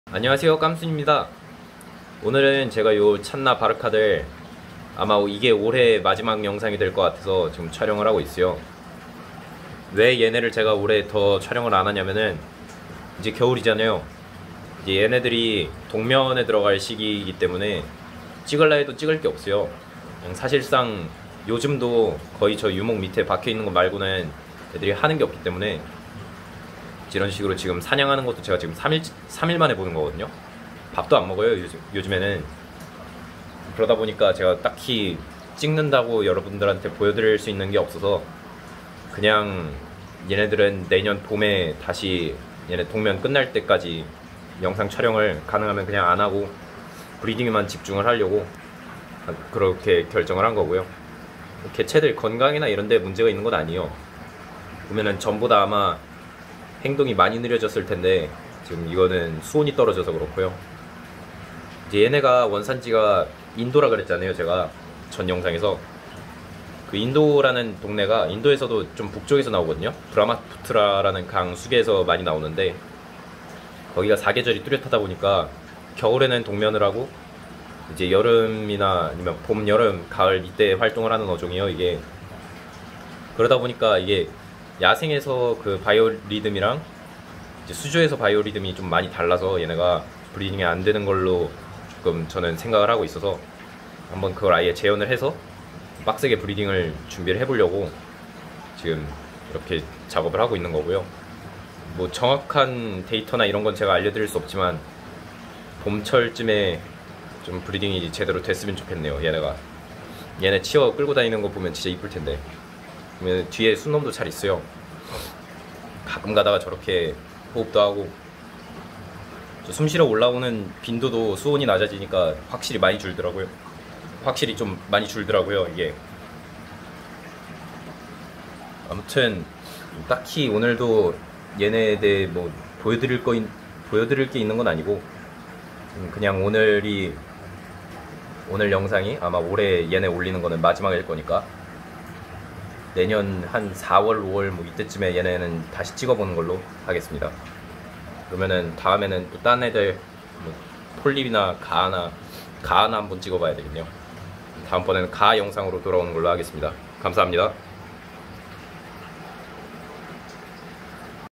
올해 마지막 가물치 영상입니다
벌써 겨울이라 바르카들이 동면에 들어가기 시작했습니다.

동면에 들어간 이후에는 먹이를 먹거나 플레어링을 하는 등 찍을 만 한 행동을 거의 하지 않기 때문에 올해 바르카 영상은 이번이 마지막이 될 것 같습니다.

내년 봄에 이 녀석들과 좋은 소식을 들고 와보도록 하겠습니다.